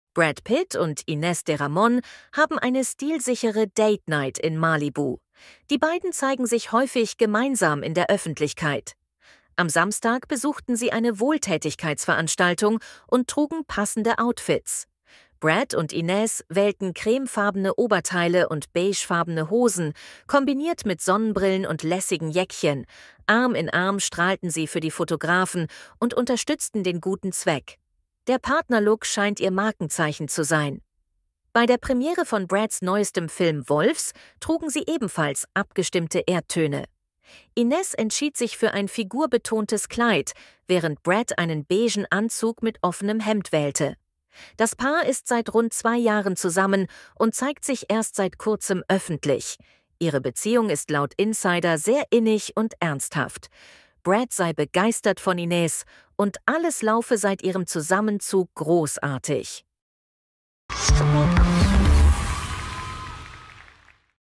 Brad Pitt und Inés de Ramón strahlen bei einer Date Night
Brad Pitt (60) und Inés de Ramón (31) zeigen sich mittlerweile regelmäßig gemeinsam in der Öffentlichkeit. Am Samstag ging es für den Schauspieler und seine Partnerin zu einer Wohltätigkeitsveranstaltung in Malibu, auf der sich die beiden wieder passend zueinander kleideten. Der Ex von Angelina Jolie (49) und Inés trugen beide cremefarbene Oberteile, beigefarbene Hosen, Sonnenbrillen und jeder ein Jäckchen, das sie lässig in ihren Händen hielten. Arm in Arm strahlten die Turteltauben in die Kameras und ließen sich für den guten Zweck ablichten, wie Bilder zeigen, die People vorliegen.

Der Partnerlook scheint für Brad und Inés mittlerweile eine Art Markenzeichen geworden zu sein. Bei der Premiere von Brads neuestem Film "Wolfs" in Los Angeles zeigten sich der 60-Jährige und die Schmuckdesignerin bereits in ähnlichen Erdtönen. Die 31-Jährige entschied sich an dem Abend für ein figurbetontes Kleid mit asymmetrischen Trägern. Ihr Partner trug ebenfalls einen Anzug in Beige, den er mit einem weit aufgeknöpften cremefarbenen Hemd kombinierte.

Brad und Inés gehen bereits seit rund zwei Jahren gemeinsam durchs Leben. Ihren ersten Pärchenauftritt in der Öffentlichkeit feierten die beiden allerdings erst vor wenigen Wochen. Die Liebe zwischen Brad und Inés soll aber sehr innig sein, wie ein Insider gegenüber People Anfang des Sommers verriet: "Brad liebt so viele Dinge an ihr. Sie ist großartig für ihn. Es ist mehr eine ernsthafte Beziehung als nur ein Date. Seit sie zusammengezogen sind, ist alles großartig." (pam/pam)